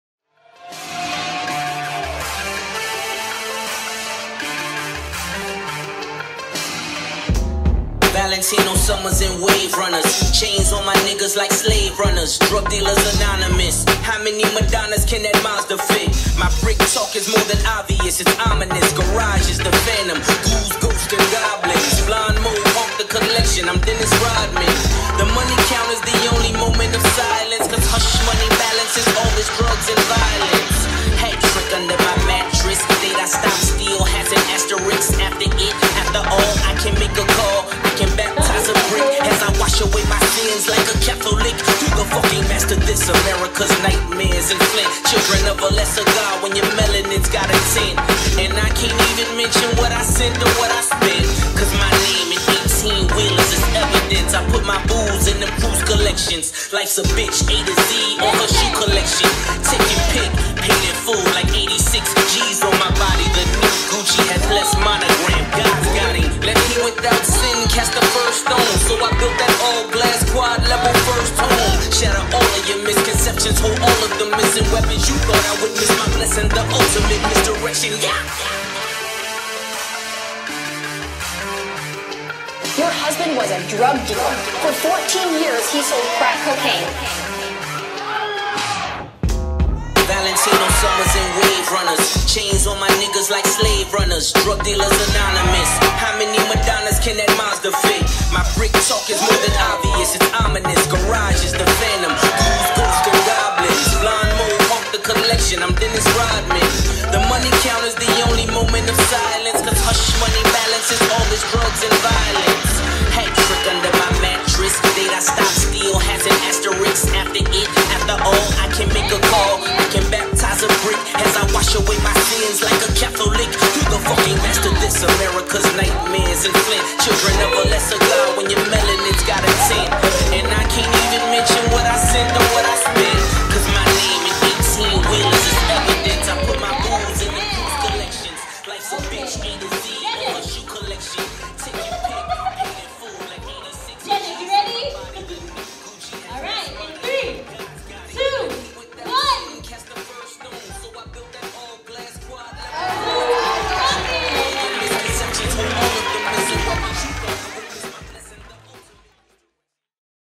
Toppy (TH) vs SY (SG) | Top8 | The Ladies Call Vol. 4 Singapore | RPProds
The Ladies Call Vol. 4 Singapore
Organized by Xiao Mei

The Ladies' Call a.k.a TLC ; presenting to you a 1v1 Hip Hop battle JUST FOR THE LADIES! Calling out to all the ladies in the street dance scene! A battle where the ladies call the shots! As you know there are always more guys joining and representing in the Hip Hop Battles therefore TLC aims to put the focus on the ladies and to create a platform for ladies to represent! We are uniting all the girl power in the scene cause it's time for the female Hip Hop dancers to step up!

Judges:
Eda (Singapore)
Baby Sleek (Korea)
Martha (Sweden)

DJ: Bolo
Emcee: Madhu & Lum Lum

Filmed by Wen Jun Sam